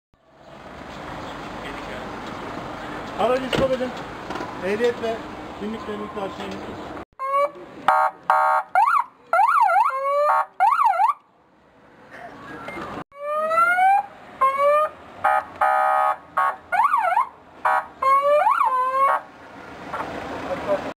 Denizli'de aracına siren sesi koyan şahısa ceza - Denizli Haberleri - HABERDENİZLİ.COM
Denizli'de polisin okul çevrelerinde yaptığı uyuşturucu ve güvenlik denetimi sırasında ilginç görüntüler de yaşandı. Denetim esnasında otomobilinde polis, ambulans ve itfaiye sesi çıkarabilen bir siren takılı sürücüye 108 TL para cezası kesildi.

HABERDENİZLİ.COM

Denizli İl Emniyet Müdürlüğü, kentte bulunan bazı ortaokul ve liselerin çevrelerinde uyuşturucu madde satışının önüne geçmek adına denetim yaptı.

Narkotik Suçlarla Mücadele Şube Müdürlüğü ve Kaçakçılık ve Organize Suçlarla Mücadele Şube Müdürlüğü ekiplerinin düzenlediği denetimlere özel eğitimli narkotik köpekleri de katıldı.

108 TL CEZA YEDİ

Eş zamanlı olarak 6 okul çevresinde düzenlenen uygulamada, şüpheli şahıslar ve otomobiller  durdurularak didik didik arandı.

Ekipler, Rasim Özkardeşler Ortaokulu çevresinde çevirdikleri 20 R 9165 plakalı otomobilin araması sırasında araçta kornanın yanı sıra sirende takılı olduğunu fark etti.

Polis ekiplerinin otomobildeki incelemesi sonucu, takılı bulunan sirenden polis ambulans ve itfaiye seslerinin çalınabildiği belirlendi.

Sireni söktürmesi için uyarılan sürücüye ise olay yerine çağrılan trafik polisleri tarafından cezai işlem uygulandı.